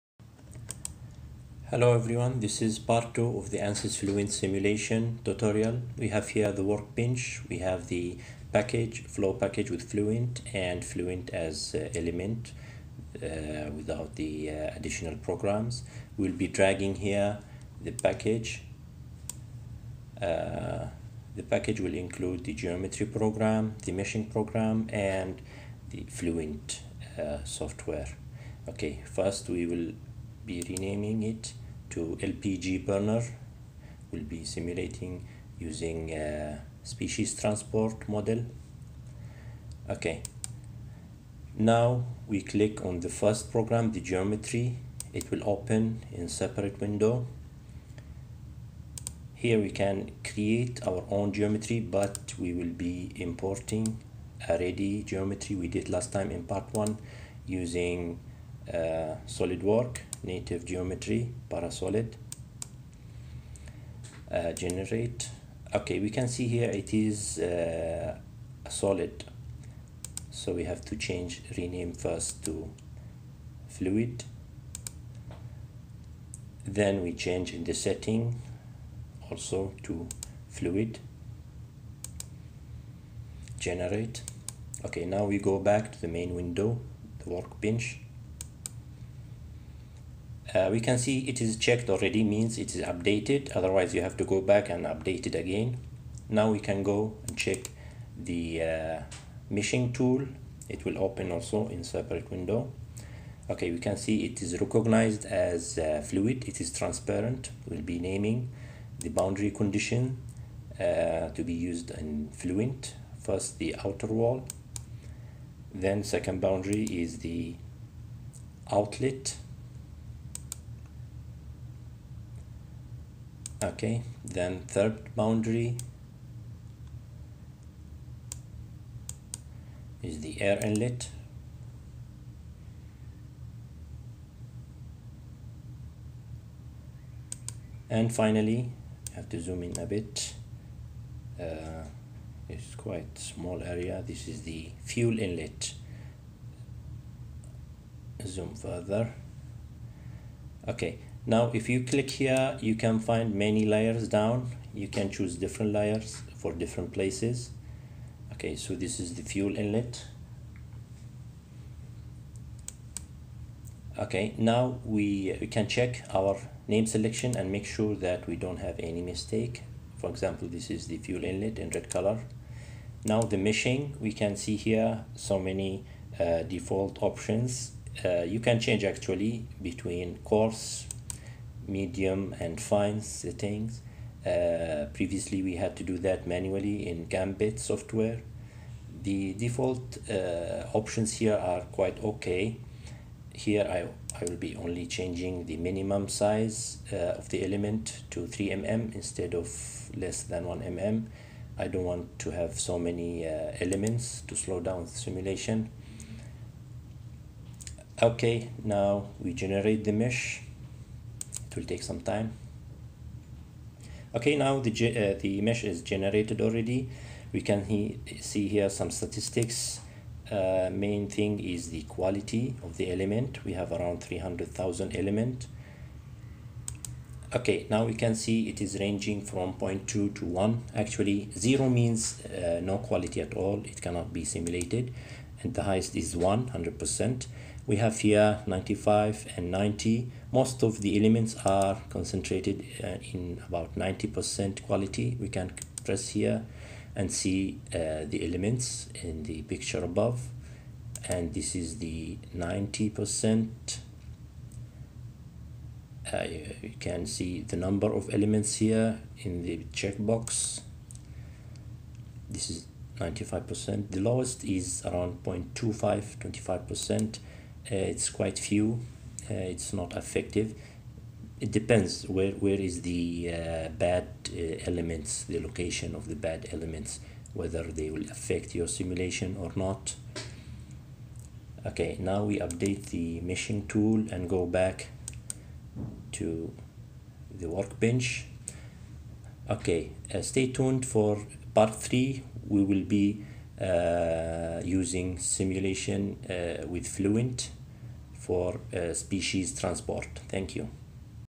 Part 2: ANSYS-Fluent workbench tutorial for gas burner with air swirler
In part 2, the process of importing the burner geometry from solidworks to the Workbench, geometry meshing process and mesh quality check are all illustrated step by step in details. You can check how the geometry was created in solidworks in part 1: